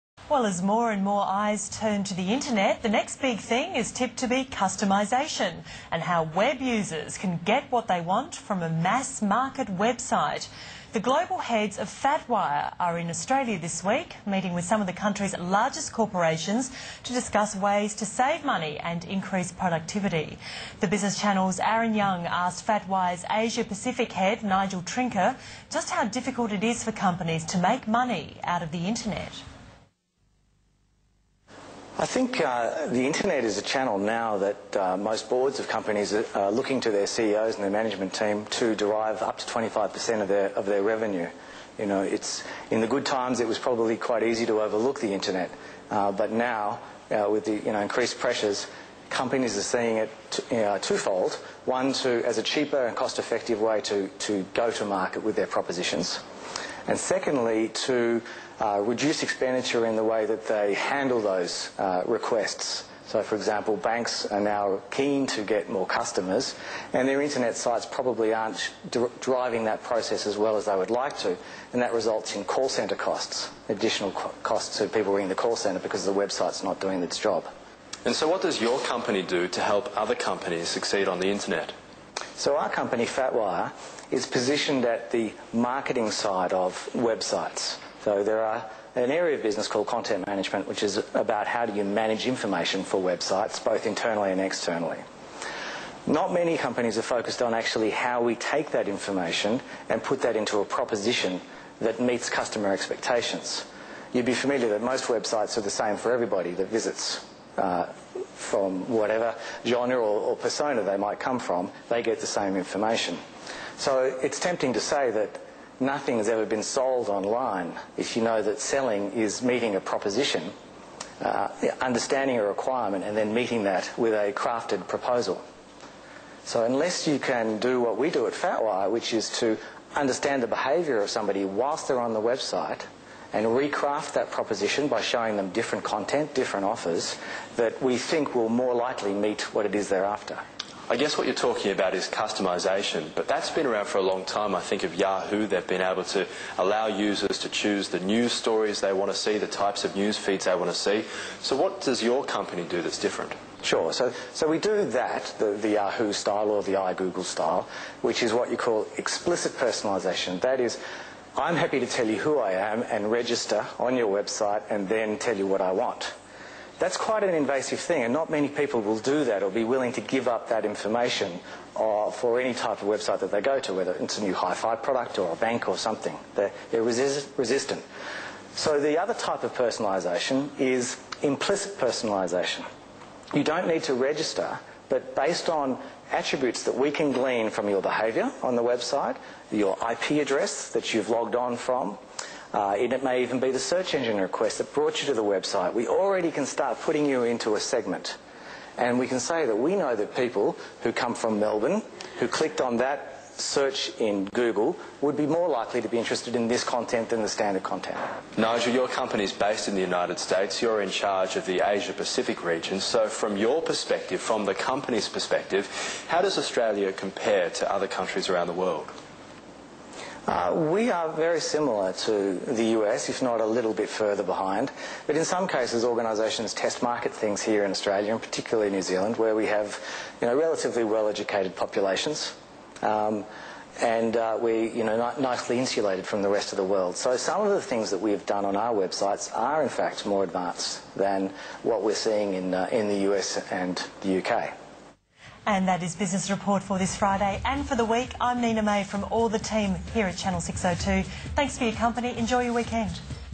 Sky Business News FatWire story May 15th
Sky Business News Interview FatWire's Nigel Trinca, VP Asia Pacific about how businesses can best use the web to convert browsers into desirable business actions.